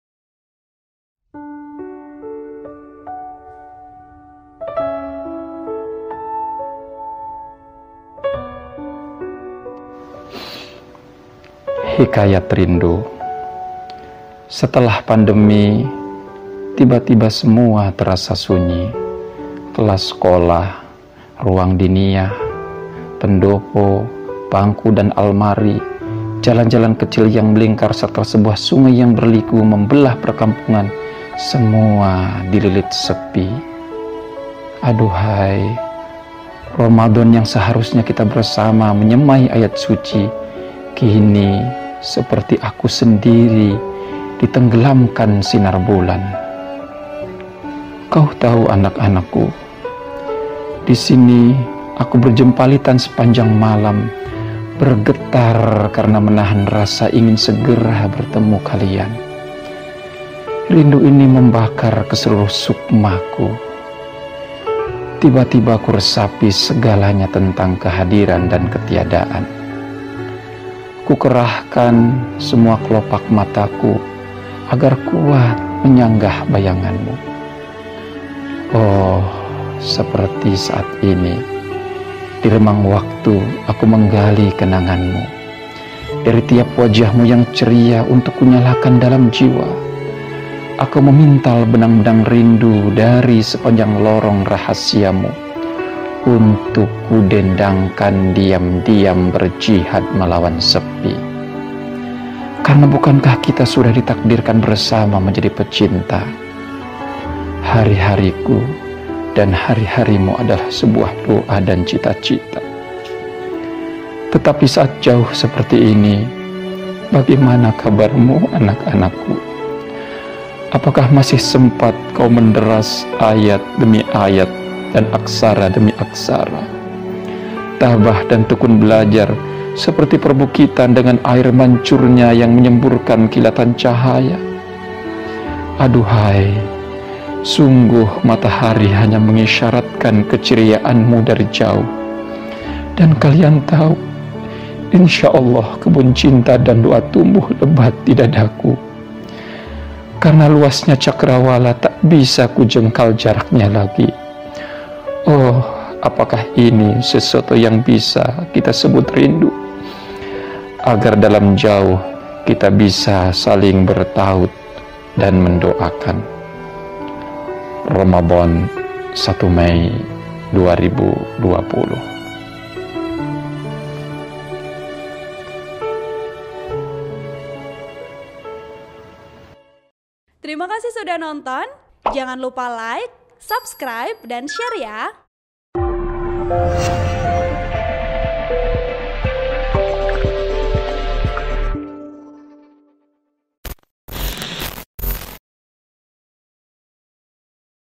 Dibacakan hingga Tersedu-sedu, Puisi Kerinduan Kiai kepada Santri di Tengah Pandemi Corona
TRIBUN-VIDEO.COM - Pandemi COVID-19 memaksa penghentian seluruh aktivitas belajar mengajar tak terkecuali di Lembaga Pendidikan Pondok Pesantren.

Pesantren-pesantren yang biasanya ramai dengan kegiatan pengajian kitab kuning selama Ramadan, saat ini sepi karena santri-santri dipulangkan ke kampung halaman bahkan sebelum datangnya bulan Ramadan.

Tiadanya aktivitas di Pondok  Pesantren ini membuat Pengasuh Pesantren Bina Insan Mulia, Cirebon, KH Imam Jazuli, bersedih dan menumpahkan kesedihannya lewat sebuah puisi yang cukup viral beredar di kalangan santri-santriwati.

Berikut petikan puisi tersebut:

HIKAYAT RINDU
KH Imam Jazuli

Setelah pandemi, tiba-tiba semua terasa sunyi
kelas sekolah, ruang diniyah, pendopo, bangku dan almari
jalan-jalan kecil yang melingkar, serta sebuah sungai
yang berliku membelah perkampungan semua dililit sepi
Aduhai, Ramadhan yang seharusnya kita bersama menyemai ayat suci
kini seperti aku sendiri ditenggelamkan sinar bulan

Kau tahu, anak-anakku, disini aku berjempalian sepanjang malam
bergetar karena menahan rasa ingin segera bertemu kalian?
rindu ini membakar ke seluruh sukmaku tiba-tiba
kuresapi segalanya tentang kehadiran dan ketiadaan
kukerahkan semua kelopak mataku agar kuat menyanggah bayanganmu

Oh, Seperti saat ini, di remang waktu aku menggali kenanganmu
dari tiap wajahmu yang ceria Untuk kunyalakan dalam jiwa
aku memintal benang rindu dari sepanjang lorong rahasiamu
untuk kudendangkam diam-diam, berjihad melawan sepi
karena bukankah kita sudah ditakdirkan bersama menjadi pecinta?
hari-hariku dan hari-harimu adalah sebuah doa dan cita-cita

Tetapi saat jauh seperti ini? Bagaimana kabarmu, anak-anakku?
apakah masih sempat kau mendaras ayat demi ayat dan aksara demi aksara?
tabah dan tekun belajar seperti perbukitan dengan air mancurnya
yang menyemburkan kilatan cahaya?

Aduhai, sunggguh matahari hanya mengisyaratkan kecerianmu dari jauh
dan kalian tahu, insyaallah kebun cinta dan doa tumbuh lebat di dadaku
karena luasnya cakrawala, tak bisa kujengkal jaraknya lagi
Oh, apakah ini sesuatu yang bisa kita sebut rindu?
agar dalam jauh kita bisa saling bertaut dan mendoakan?

Cirebon, 1 Mei 2020